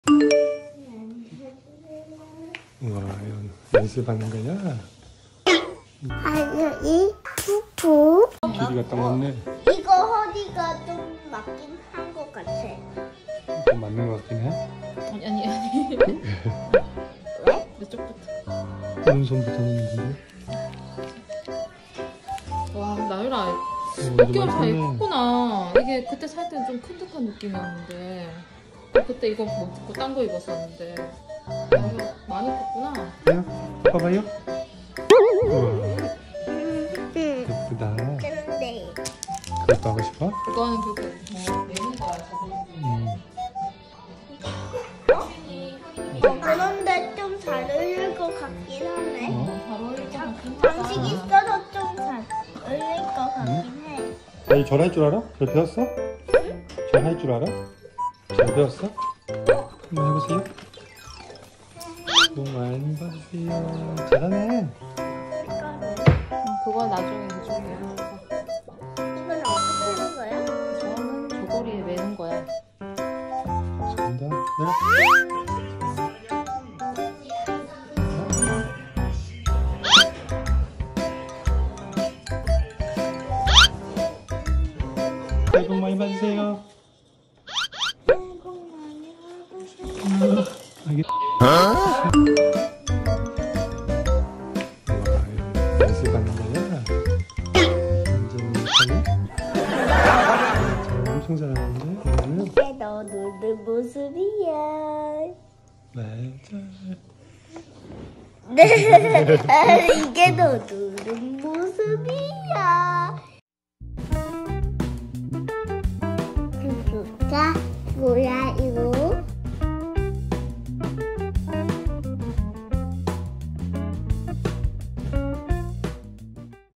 한복 입고 절 연습하기 / Korean traditional cloths Hanbok
작년에 한복을 입고서 절을 연습하던 영상입니다😊
시간은 좀 되었지만 귀엽게 봐 주세요